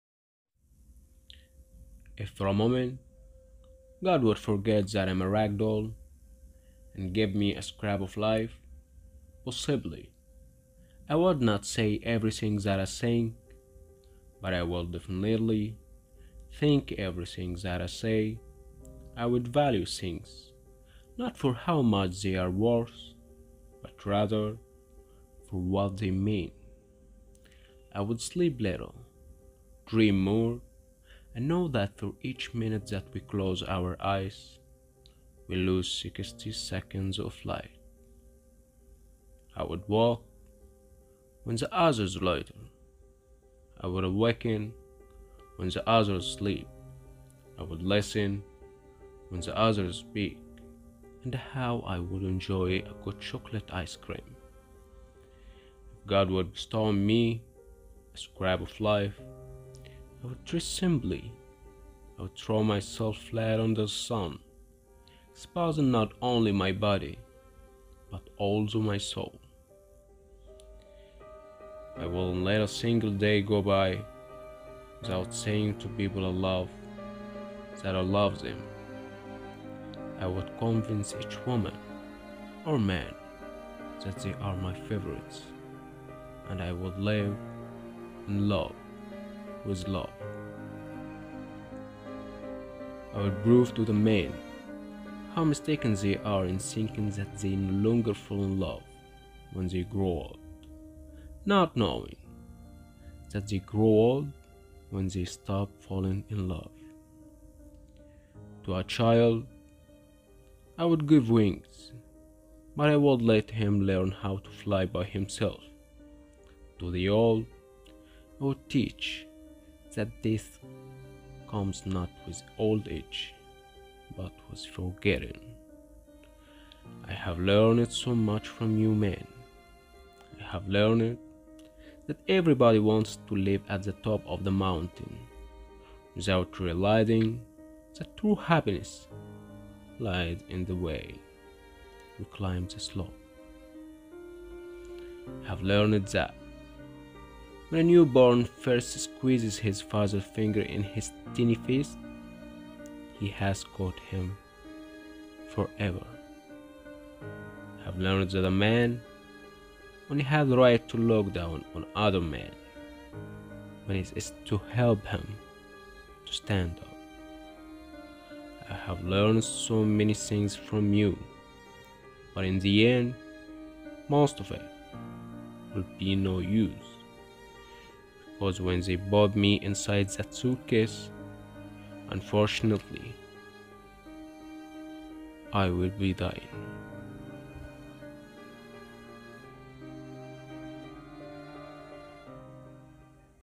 The puppet - johnny welch about Gabriel Garcia Marquez
If for a moment God would forget that I am a rag doll and give me a scrap of life, possibly I would not say everything that I think, but I would definitely think everything that I say.

I would value things not for how much they are worth but rather for what they mean.

I would sleep little, dream more. I know that for each minute that we close our eyes we lose sixty seconds of light.

I would walk when the others loiter; I would awaken when the others sleep.

I would listen when the others speak, and how I would enjoy a good chocolate ice cream.

If God would bestow on me a scrap of life, I would dress simply, I would throw myself flat under the sun, exposing not only my body but also my soul.

I wouldn't let a single day go by without saying to people I love, that I love them.

I would convince each woman or man that they are my favourites and I would live in love with love.

I would prove to the men how mistaken they are in thinking that they no longer fall in love when they grow old-not knowing that they grow old when they stop falling in love. To a child I would give wings, but I would let him learn how to fly by himself. To the old I would teach that death comes not with old age but with forgetting. I have learned so much from you men….

I have learned that everybody wants to live at the top of the mountain without realizing that true happiness lies in the way we climb the slope.

I have learned that when a newborn first squeezes his father's finger in his tiny fist, he has caught him forever.

I have learned that a man only has the right to look down on another man when it is to help him to stand up. I have learned so many things from you, but in the end most of it will be no use because when they put me inside that suitcase, unfortunately I will be dying.